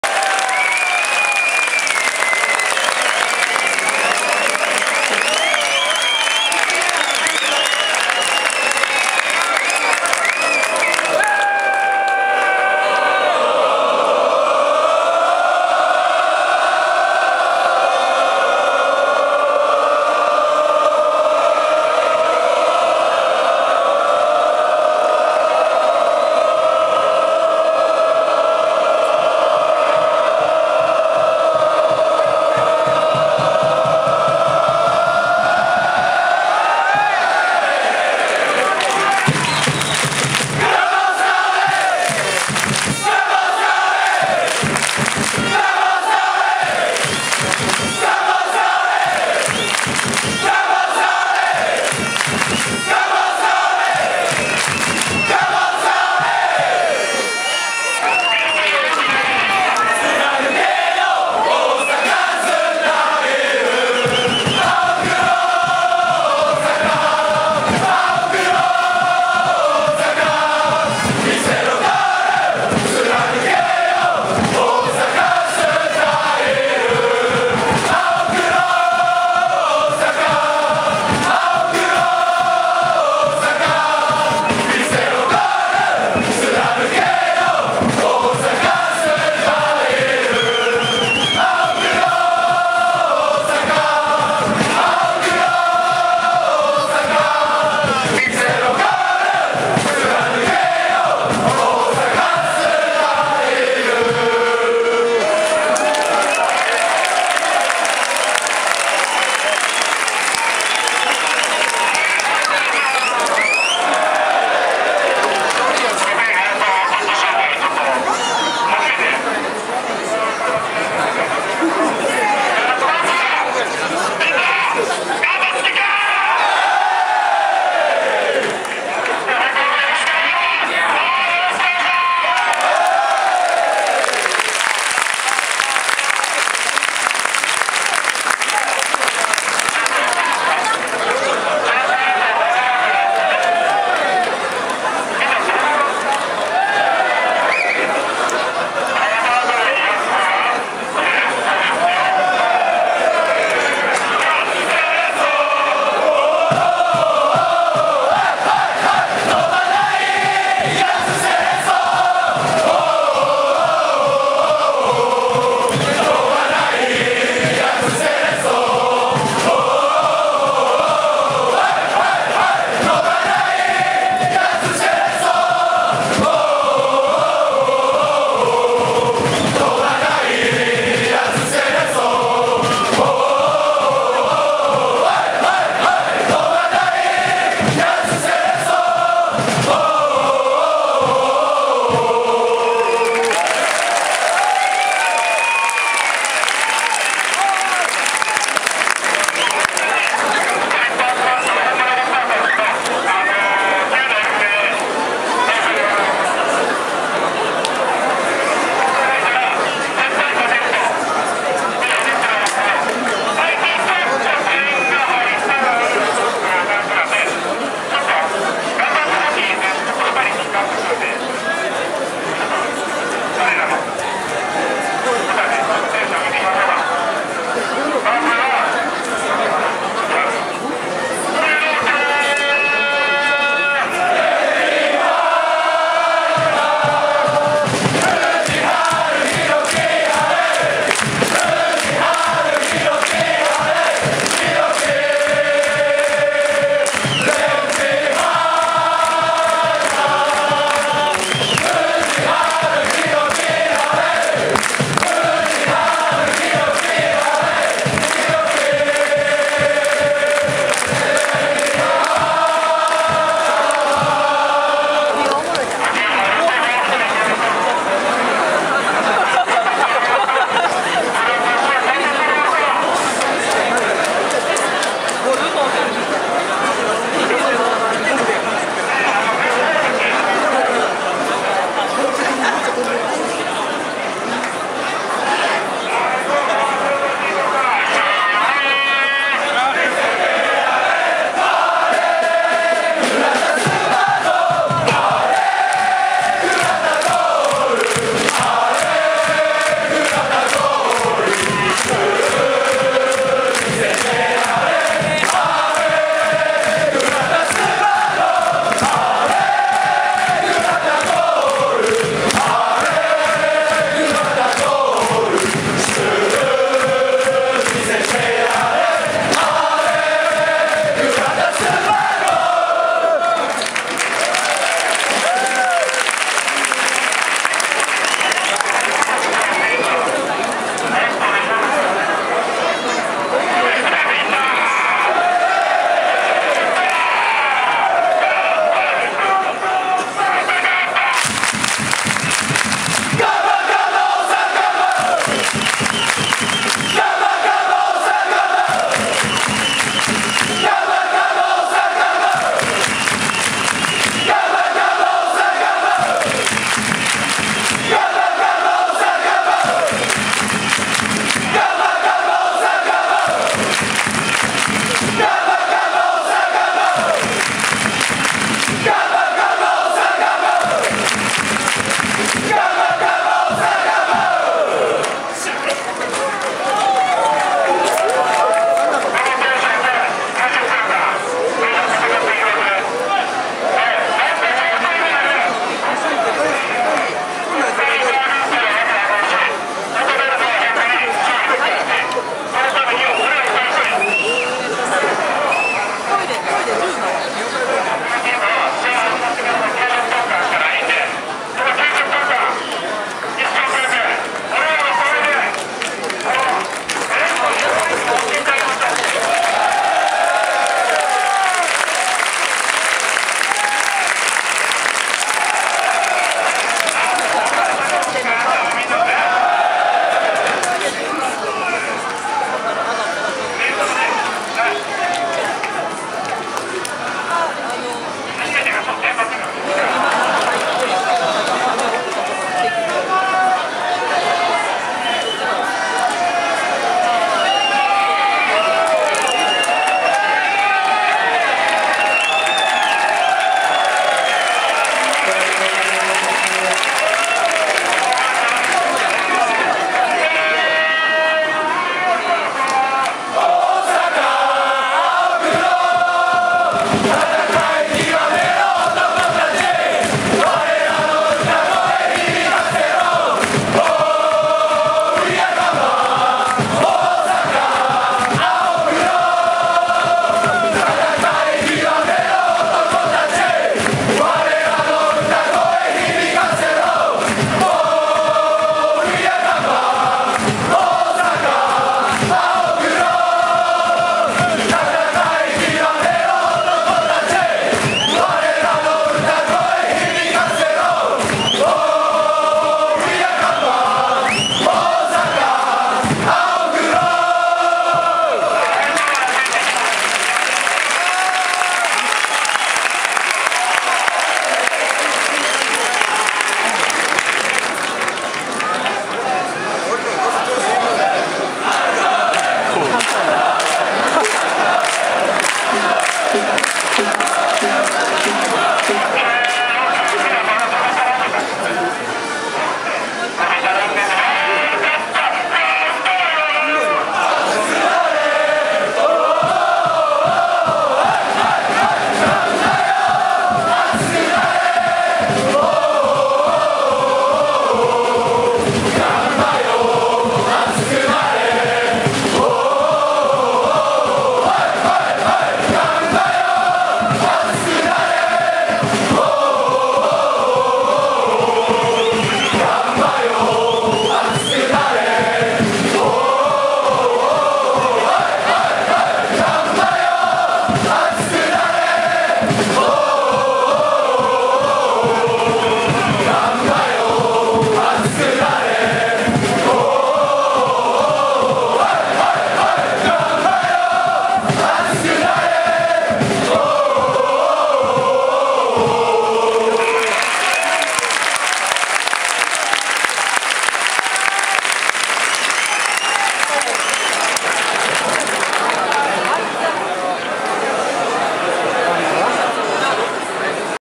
ガンバ大阪サポーター 決起集会 2014-04-12
2014年4月12日、ヤンマースタジアム長居で２年ぶりの大阪ダービー。アウェイスタンドE1ゲートコンコースに集まったガンバ大阪サポーターによる決起集会。